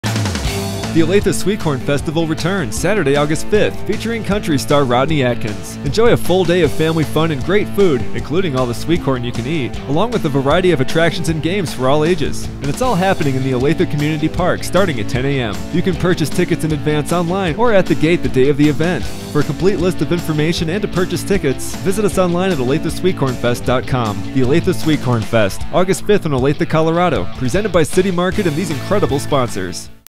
Olathe Sweet Corn Festival | Featuring Rodney Atkins
Join Us August 5th for the Olathe Sweet Corn Festival With Rodney Atkins